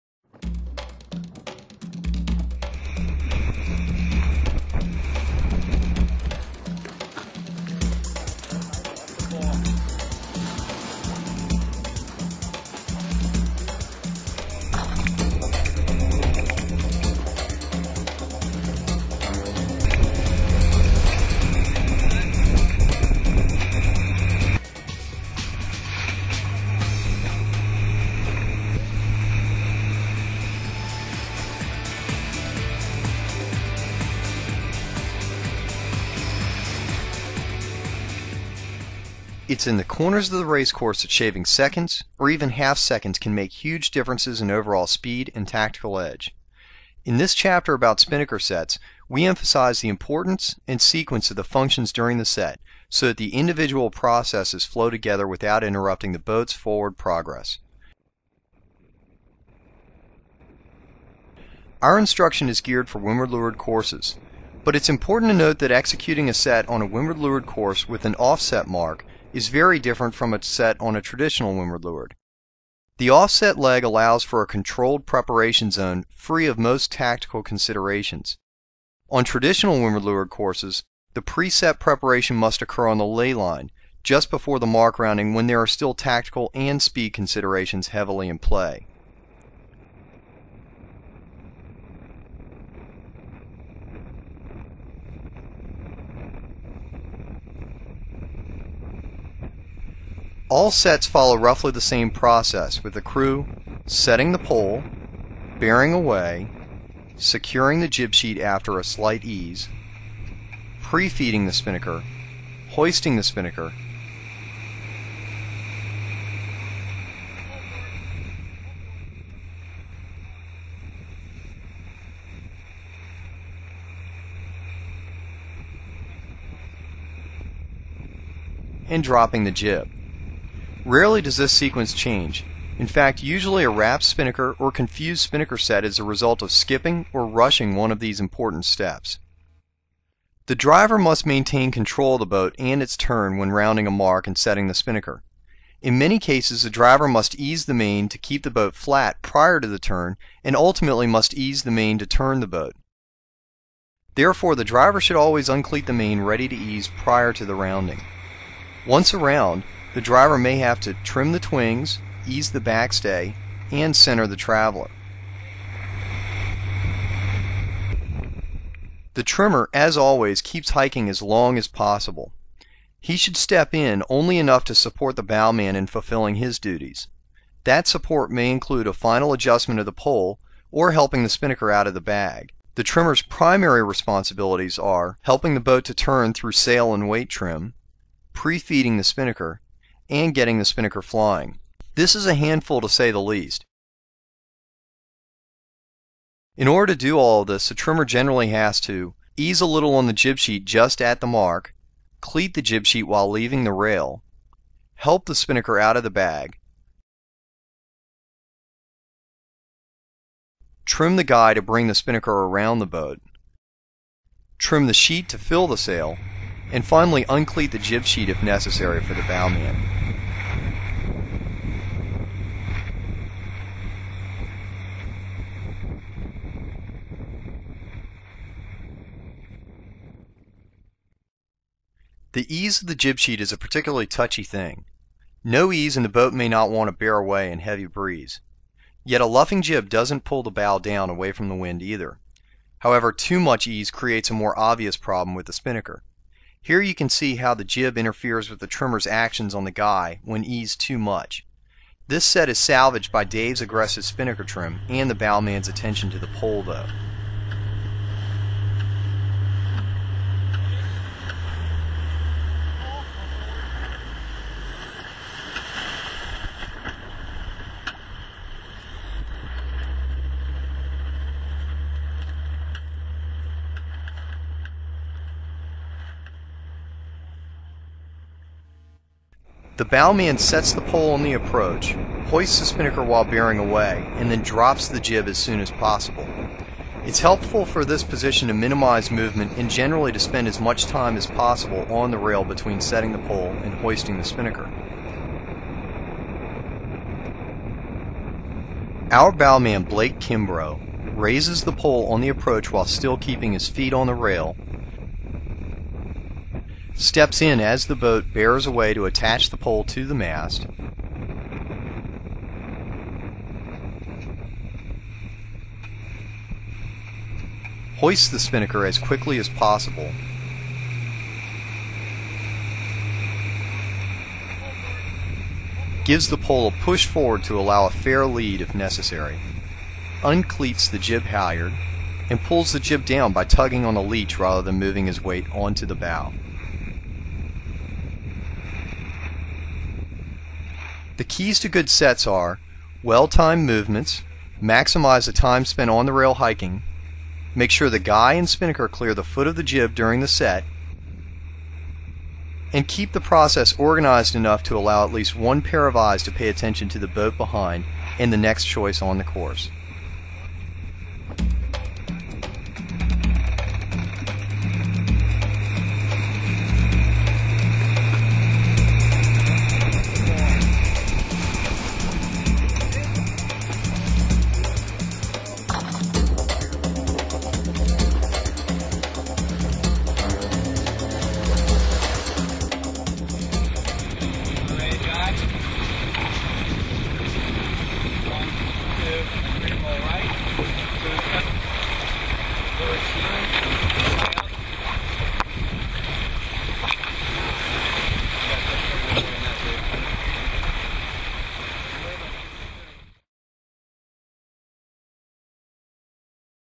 J/22 Spinnaker Sets
How to set a spinnaker on a J/22 sailboat.